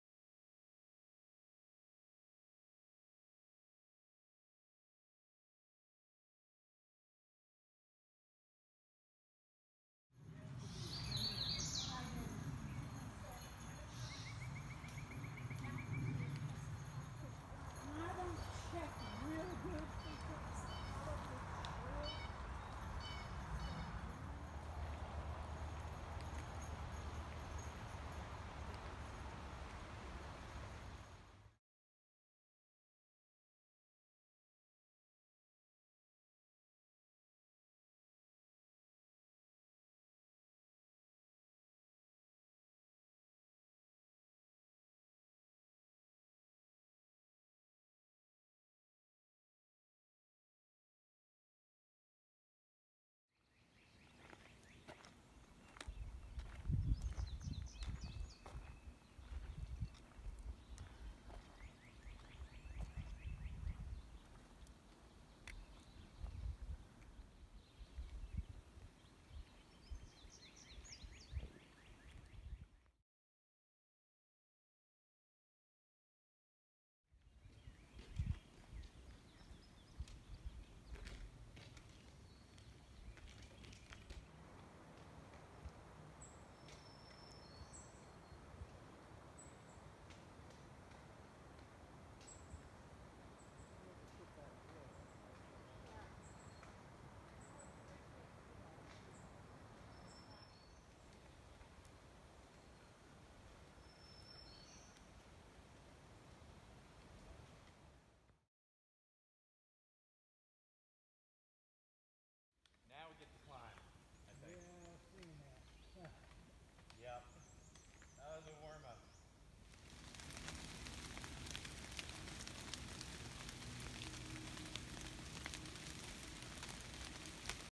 Day 49 - Appalachian Trail 2018
Day 49, 5/6/18 - Miles 428.1 to mile 441.0. Another beautiful day! We hiked around Watauga Lake and over the Watauga Dam. It rained in the afternoon.